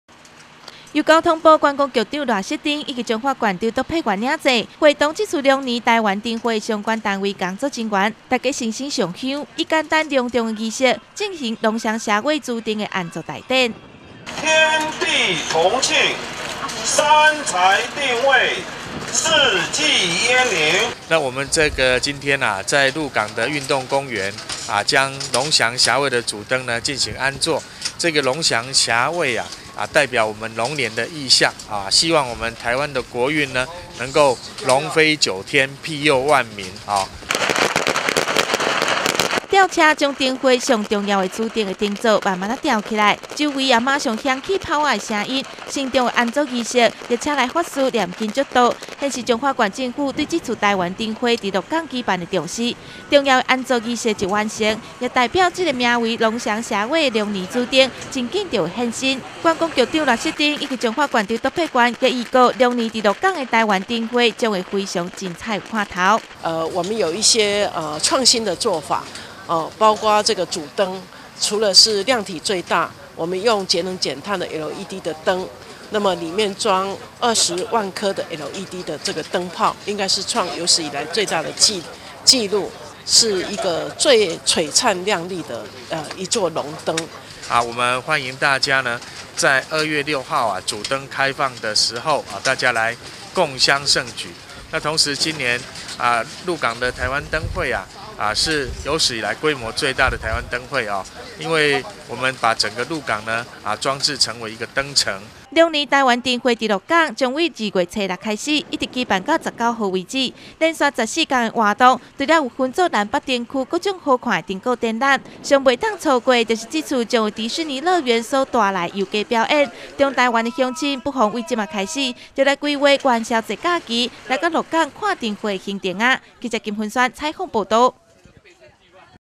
南投新聞-彰化縣臺灣燈會主燈安座
每年乎臺灣民眾看作是過年了後，最盛大好看的臺灣燈會，龍年是選置彰化縣的鹿港地區來舉辦。承辦的彰化縣政府，為著會凍將今年名為「龍翔霞蔚」的主燈，好好來打造完成，所以是提早來到主會場的鹿港體育場，由觀光局長賴瑟珍以及彰化縣長卓伯源親自領祭，舉辦了慎重的主燈安座大典。